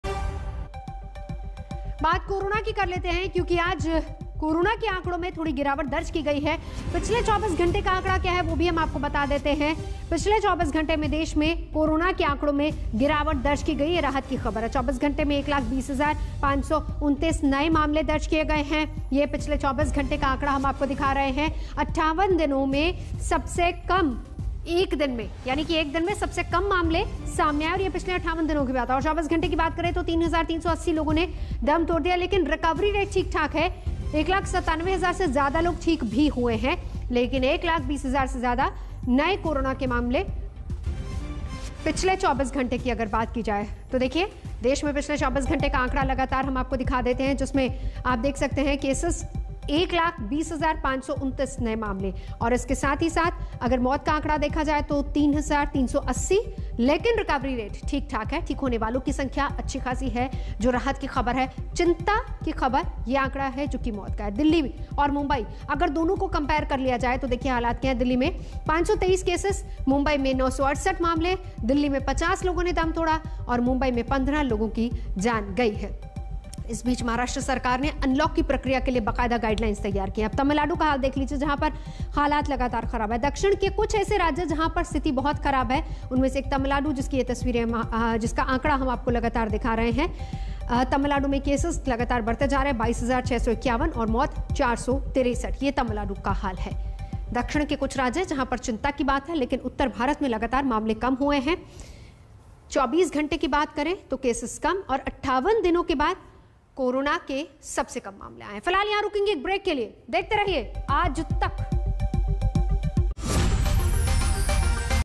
Coronavirus Outbreak In India | Covid 19 New Cases | 1,20,529 New Cases | 3,380 Deaths
Coronavirus Outbreak In Delhi : राजधानी समेत देश के बाकी शहरों में अब कोरोना का ग्राफ लगातार नीचे जा रहा है. पिछले 24 घंटे की बात करे तो राजधानी में 553 नए केस आए हैं